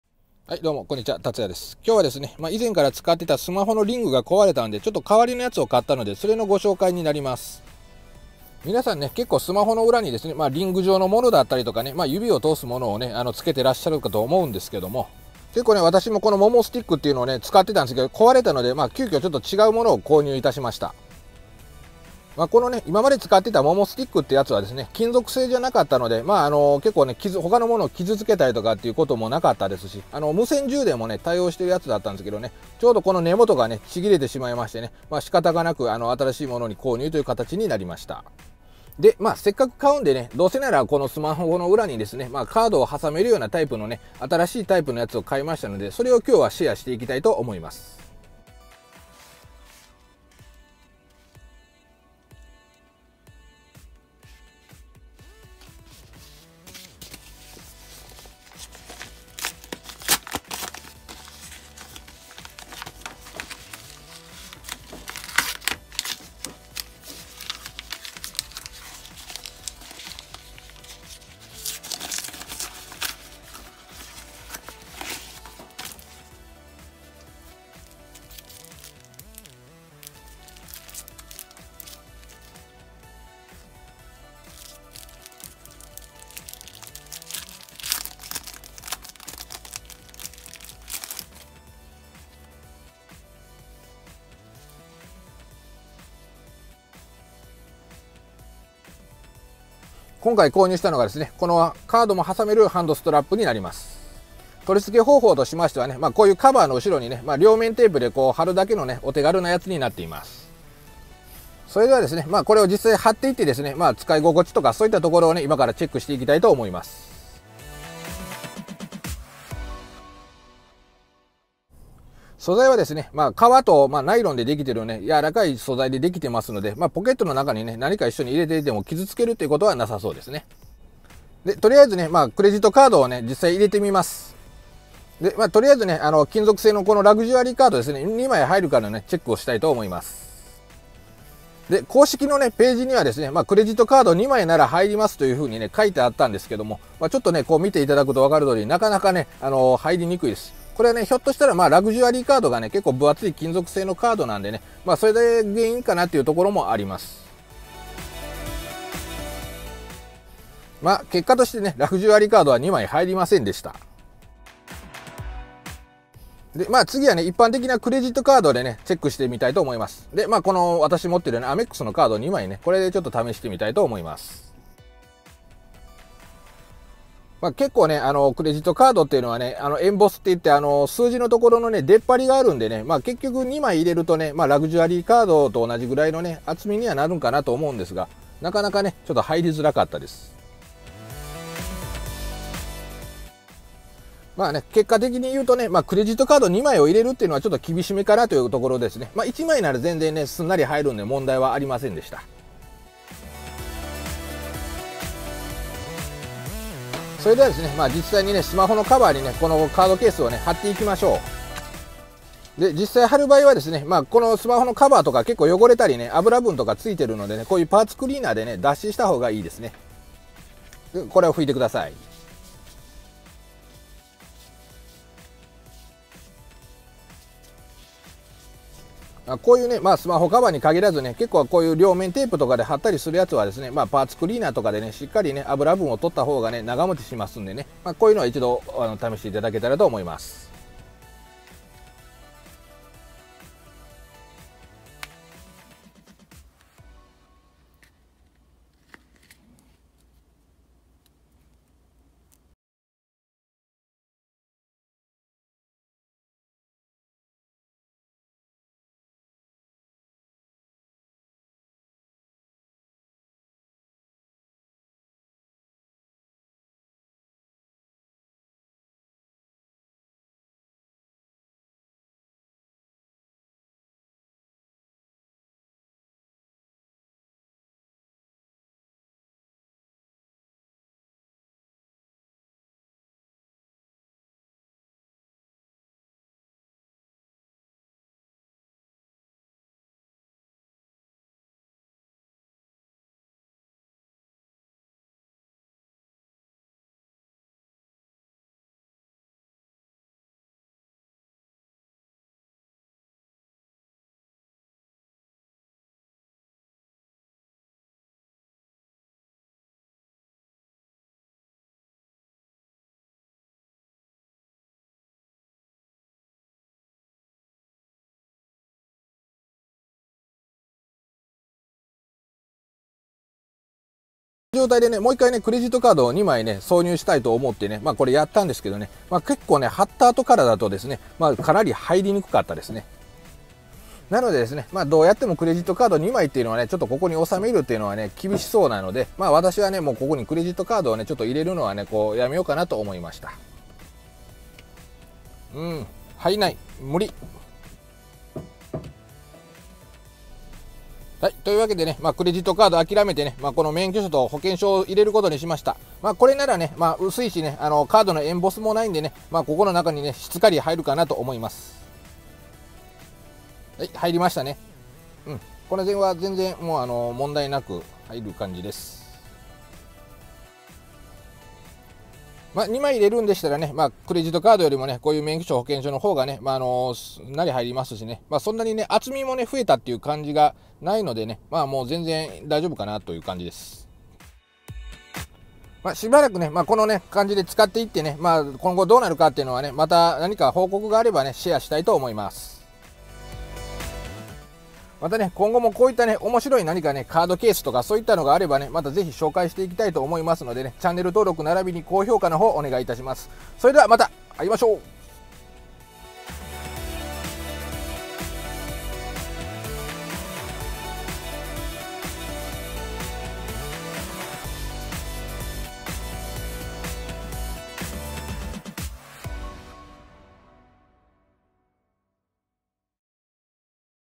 【ミニマリスト必見！】スマホに貼るだけのカードケース リング付きレビュー！【スマホリング スタンド カード入れ 落下防止 ハンドストラップ カード入れ 背面ポケット カード 財布 ミニマル】
今まで使っていたモモスティックが壊れたので、スマホの背面に貼り付けれるカードポケット付きのスマホリングを使ってみました。

これ一つで、スマホリング・カードポケット・スマホスタンドの機能を備えた便利アイテムです。

↓詳細はこちら↓
楽天
Amazon

チャンネルは毎週水曜日更新(予定

撮影機材：iPhone X
編集機材：i Movie
撮影環境：4K/60/ fps